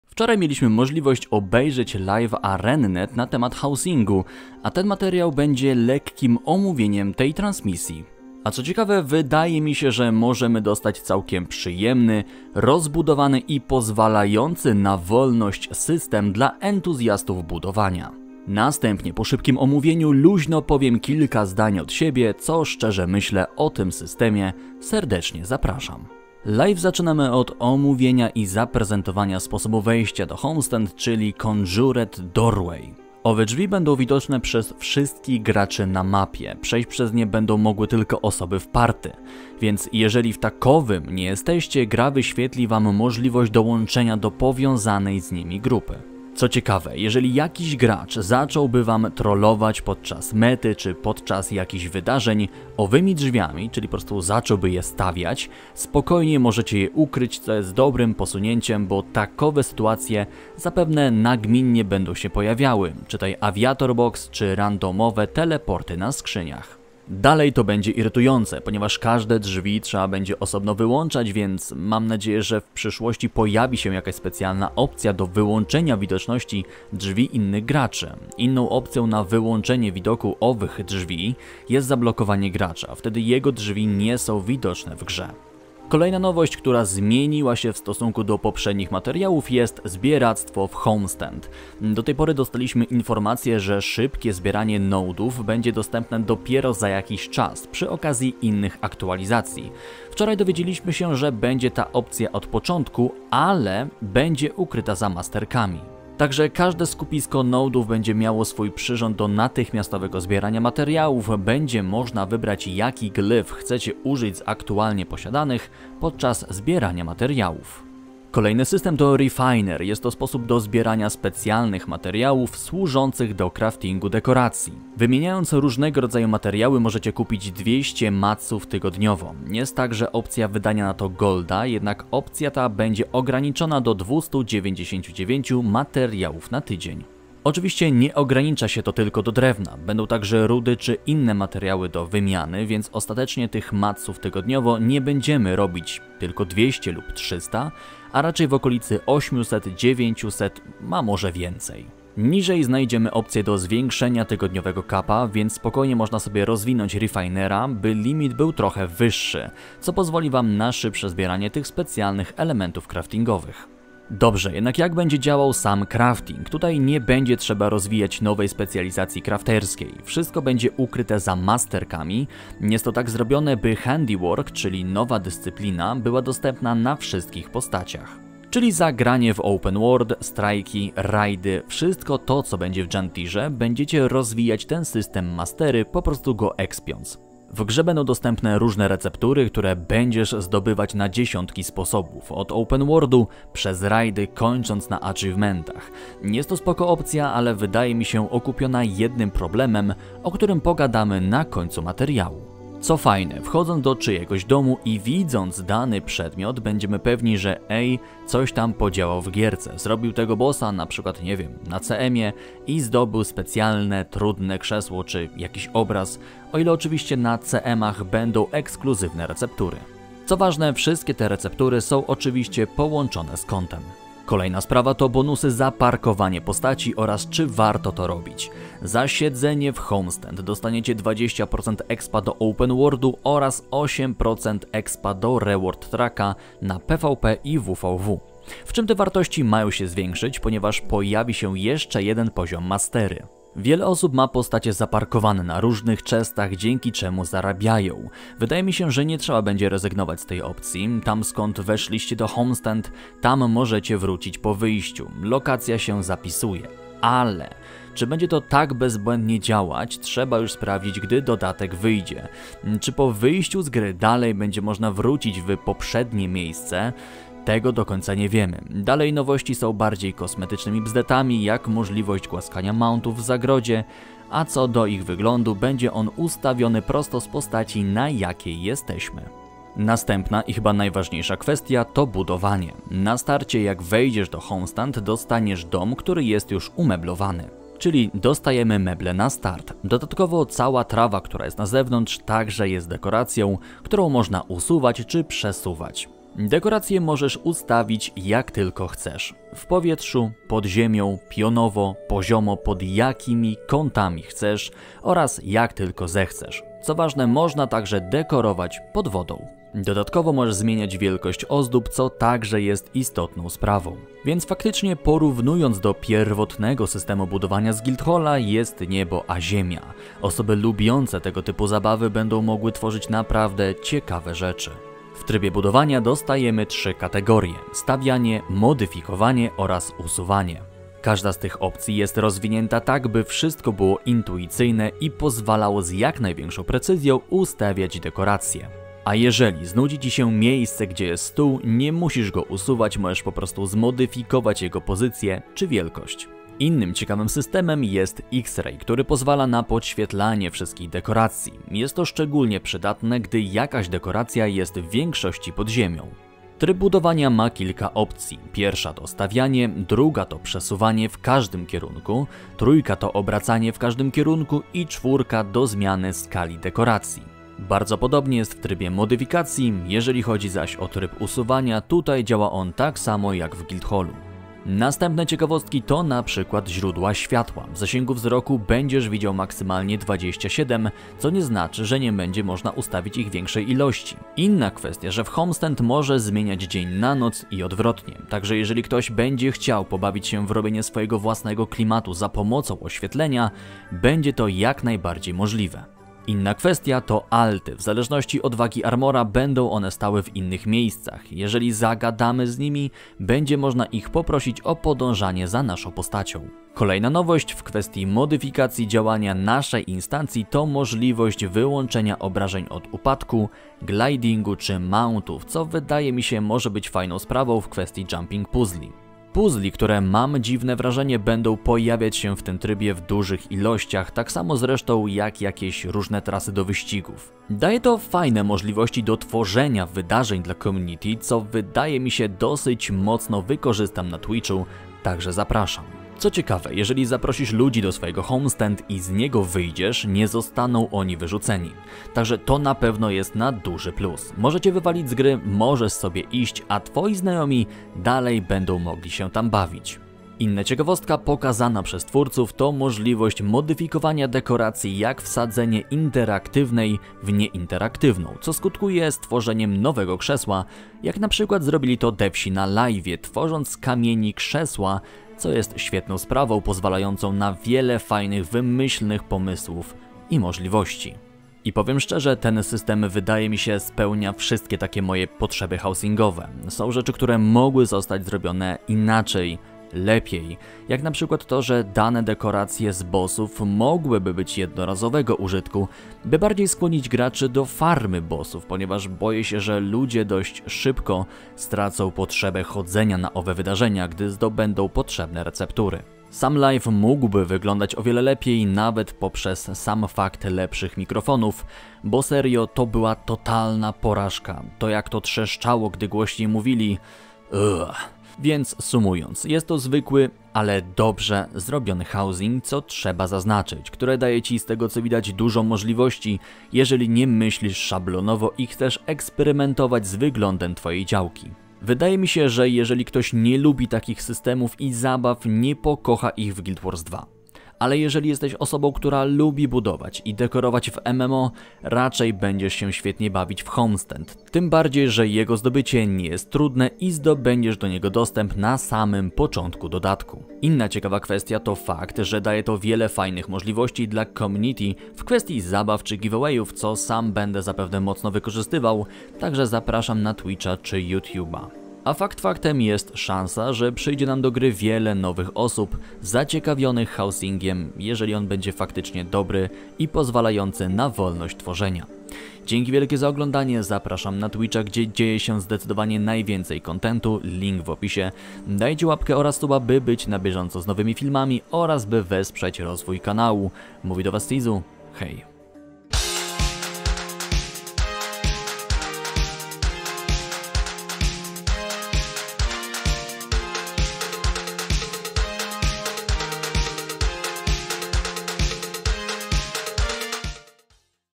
Janthir Wilds Homestand: LIVE o Housing! Czy jest dobrze?
ArenaNet, ostatnio poprowadziła Live na temat Homestand, czyli nowego systemu Housing, czy jest dobrze? A może nijako? Zapraszam do materiału!

Zajrzyj Niżej po więcej Informacji ▼